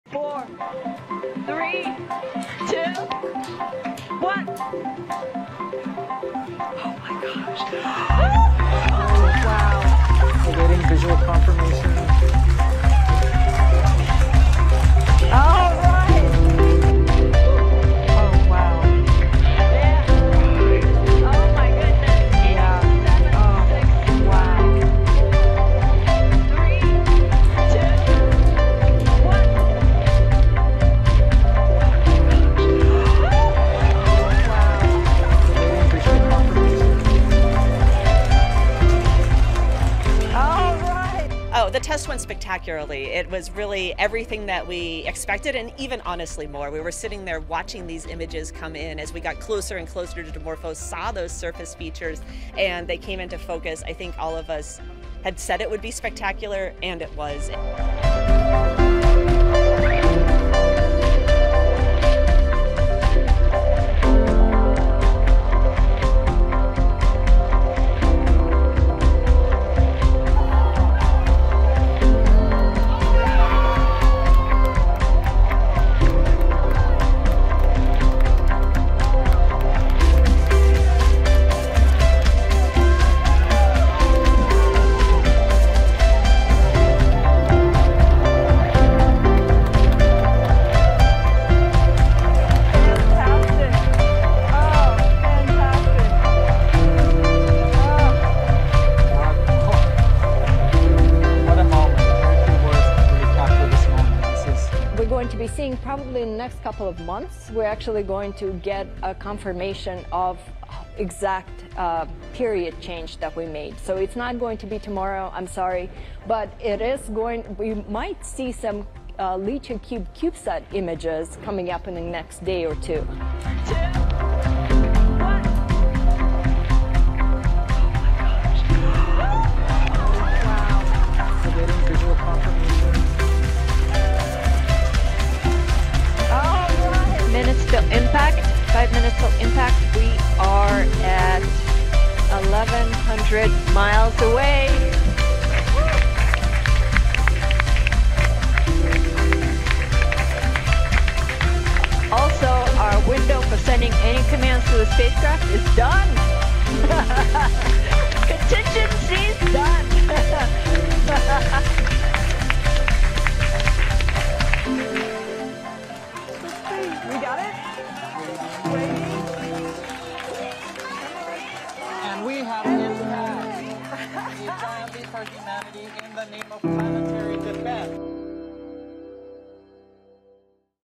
SPACE CAM: BOOM! To Save EARTH, Nasa crashes Dart spacecraft on asteroid
NASA’s DART spacecraft rammed an asteroid at blistering speed of 14,000 mph on September 27. The galactic grand slam occurred around 9.6 million kilometers away.